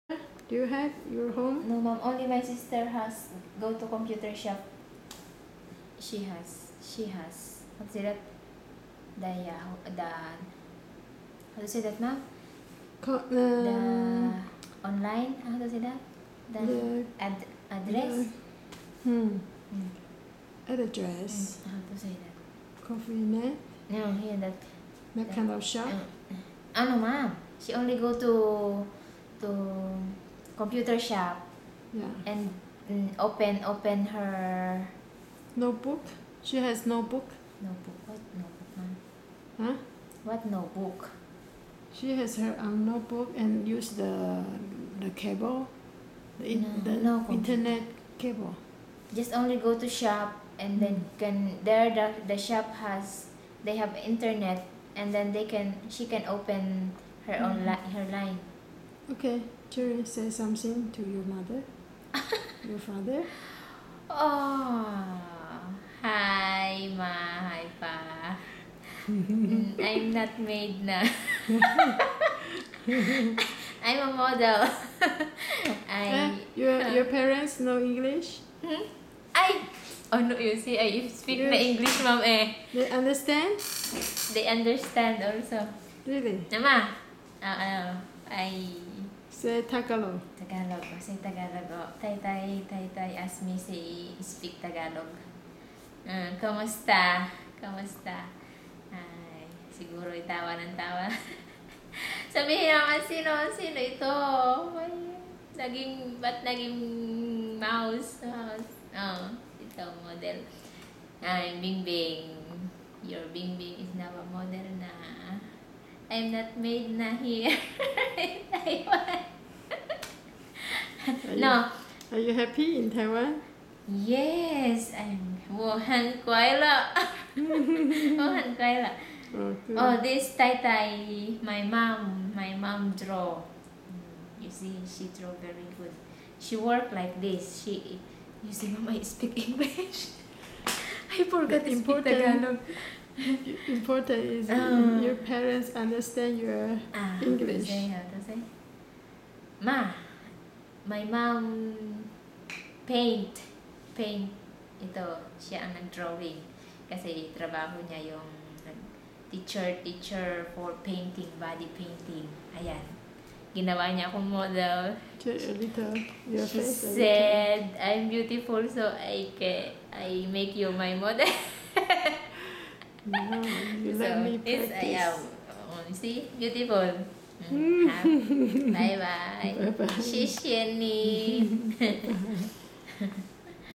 Facial painting by Hsueh Hui-ying teacher
Facial painting by Ms. Hshueh Hui-ying in 2010. The model's name is Cherry Glori from Philippines. Ms. Hsueh lives in south Taiwan, Pingtung city and teaches in Meiho university, cosmetics department.